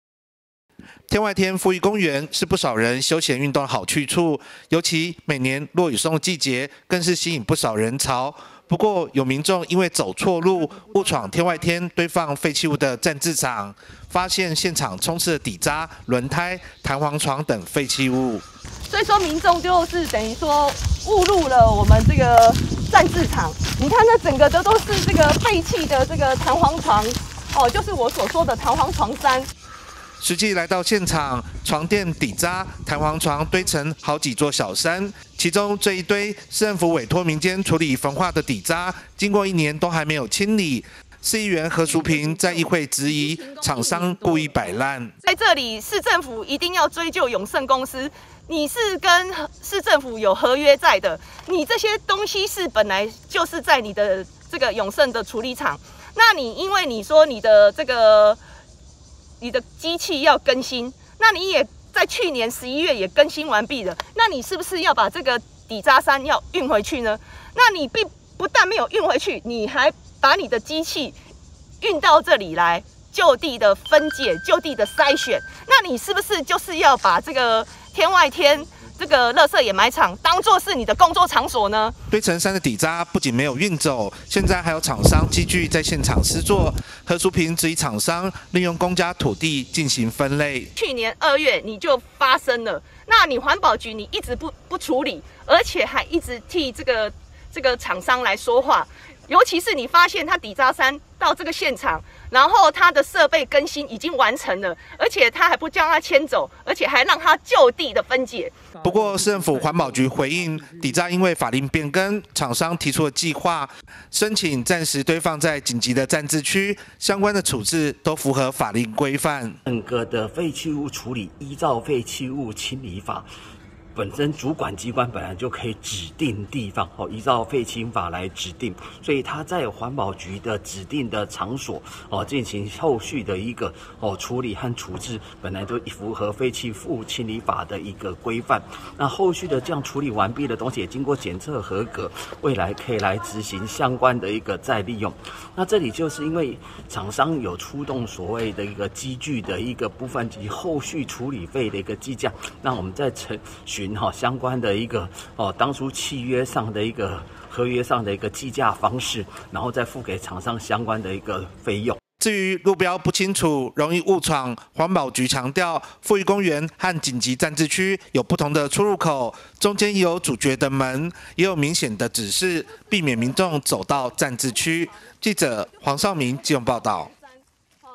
天外天｜底渣成山｜天外天推置底渣成山 何淑萍批處理廠商擺爛
(中嘉新聞記者黃紹銘/基隆採訪報導) 基市天外天掩埋場堆置了1.5萬噸底渣，市議員何淑萍在議會質詢，指廠商契約即將到期，質疑廠商存心擺爛。環保局長賴煥紘表示，廠商提出計畫，申請暫時堆置在緊急暫置區，相關處置都符合法令規範會要求業者儘速執行再利用程序。